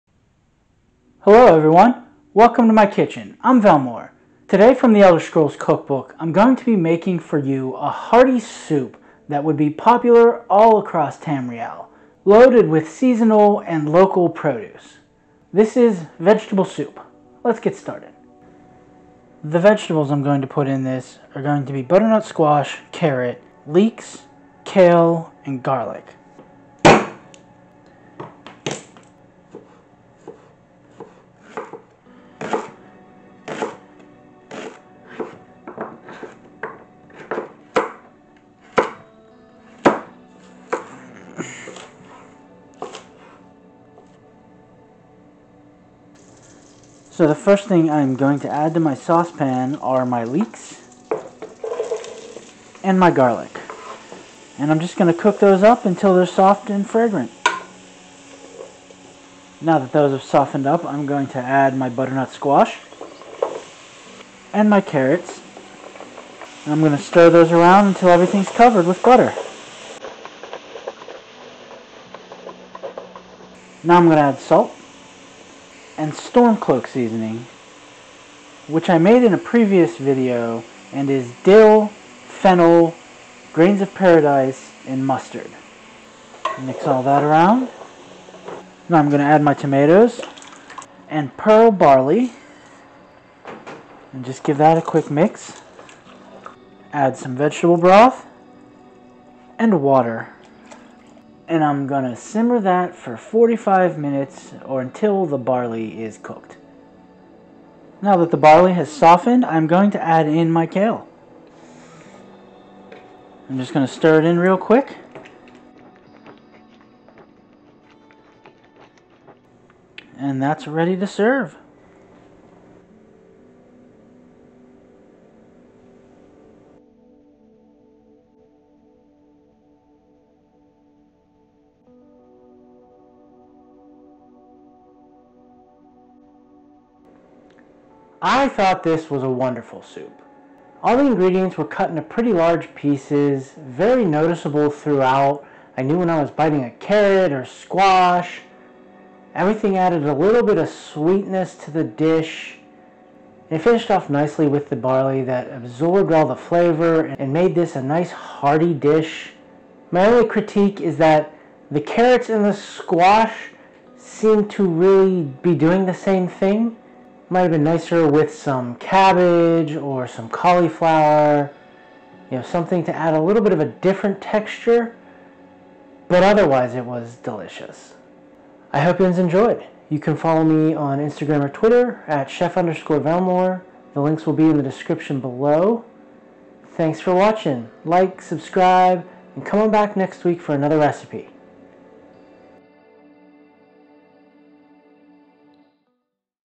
Vegetable Soup - Elder Scrolls Cookbook
From the Elder Scrolls Cookbook is Vegetable Soup. Carrots, squash, leeks and lentils make for a hearty soup full of flavor. Slightly sweet with big chunks of vegetable, it is a delicious meal that would be fantastic during summer or winter. It is a little on the short end because I got sick and wasn't able to put together something longer.